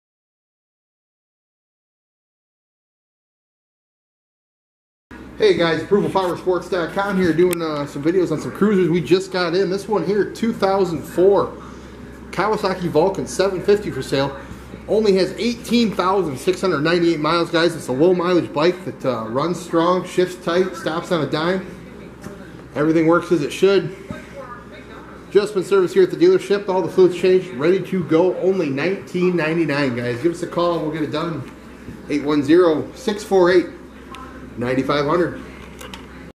2004 KAWASAKI VULCAN 750 U5560
USED 2004 KAWASAKI VULCAN 750 FOR SALE WITH ONLY 18,698 MILES! LOW MILEAGE BIKE THAT RUNS STRONG, SHIFTS TIGHT, STOPS ON A DIME! JUST SERVICED AT THE DEALERSHIP, INSPECTED, ALL FLUIDS CHANGED AND READY TO RIDE!